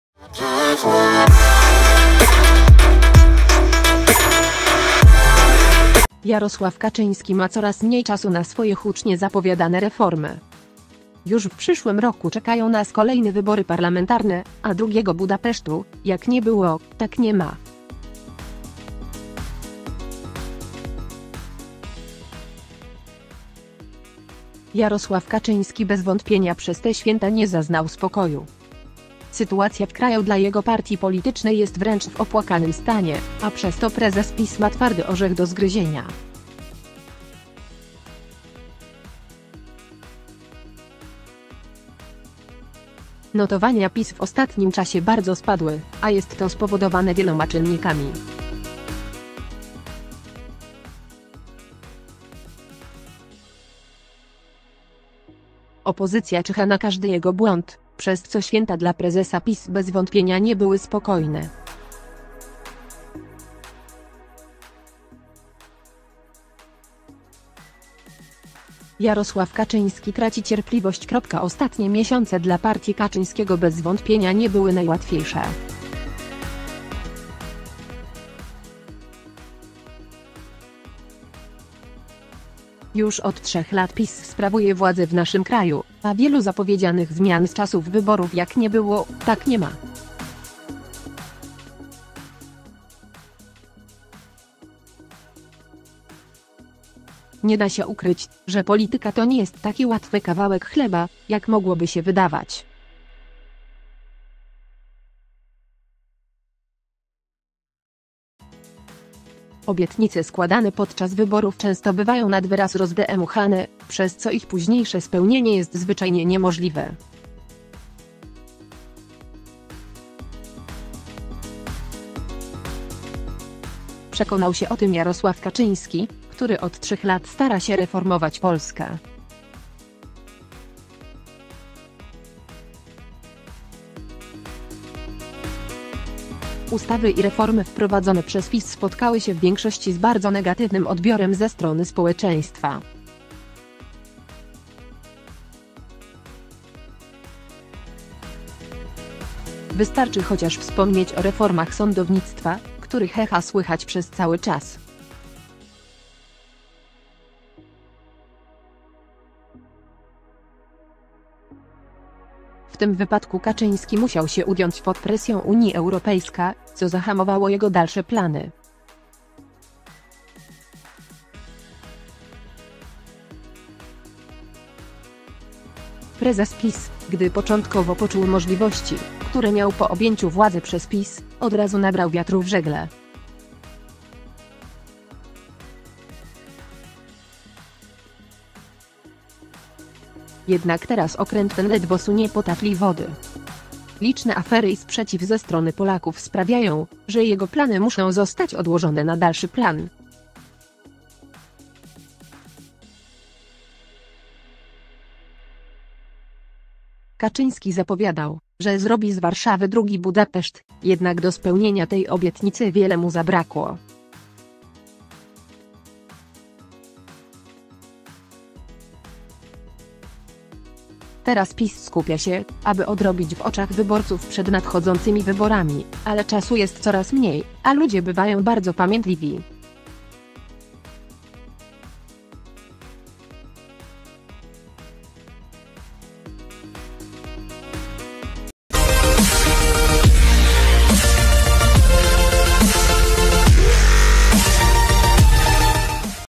Smutne święta Kaczyńskiego. Wstyd na całą Europę | Aktualności 360
Jarosław Kaczyński ma coraz mniej czasu na swoje hucznie zapowiadane reformy. Już w przyszłym roku czekają nas kolejne wybory parlamentarne, a drugiego Budapesztu, jak nie było, tak nie ma.
Jarosław Kaczyński bez wątpienia przez te święta nie zaznał spokoju. Sytuacja w kraju dla jego partii politycznej jest wręcz w opłakanym stanie, a przez to prezes PiS ma twardy orzech do zgryzienia. Notowania PiS w ostatnim czasie bardzo spadły, a jest to spowodowane wieloma czynnikami. Opozycja czyha na każdy jego błąd, przez co święta dla prezesa PiS bez wątpienia nie były spokojne.
Jarosław Kaczyński traci cierpliwość
Ostatnie miesiące dla partii Kaczyńskiego bez wątpienia nie były najłatwiejsze. Już od 3 lat PiS sprawuje władzę w naszym kraju, a wielu zapowiedzianych zmian z czasów wyborów jak nie było, tak nie ma.
Nie da się ukryć, że polityka to nie jest taki łatwy kawałek chleba, jak mogłoby się wydawać. Obietnice składane podczas wyborów często bywają nad wyraz rozdmuchane, przez co ich późniejsze spełnienie jest zwyczajnie niemożliwe. Przekonał się o tym Jarosław Kaczyński, który od 3 lat stara się reformować Polskę.

Ustawy i reformy wprowadzone przez PiS spotkały się w większości z bardzo negatywnym odbiorem ze strony społeczeństwa. Wystarczy chociaż wspomnieć o reformach sądownictwa, których echa słychać przez cały czas. W tym wypadku Kaczyński musiał się ugiąć pod presją Unii Europejska, co zahamowało jego dalsze plany.
Prezes PiS, gdy początkowo poczuł możliwości, które miał po objęciu władzy przez PiS, od razu nabrał wiatru w żagle. Jednak teraz okręt ten ledwo sunie po tafli wody. Liczne afery i sprzeciw ze strony Polaków sprawiają, że jego plany muszą zostać odłożone na dalszy plan.

Kaczyński zapowiadał, że zrobi z Warszawy drugi Budapeszt, jednak do spełnienia tej obietnicy wiele mu zabrakło. Teraz PiS skupia się, aby odrobić w oczach wyborców przed nadchodzącymi wyborami, ale czasu jest coraz mniej, a ludzie bywają bardzo pamiętliwi.